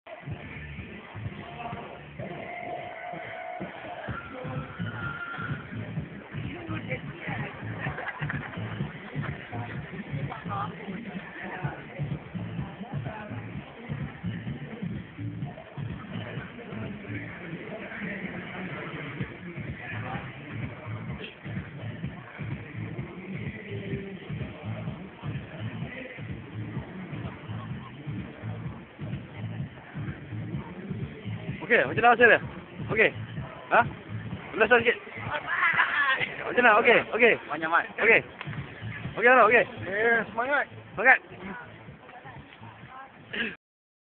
rhb unity game 2009
rhb unity game at Bayvie , Penang. Video taken during warm-up session and the three guys were Shamsul, Zulfakhri and Hairul Nazri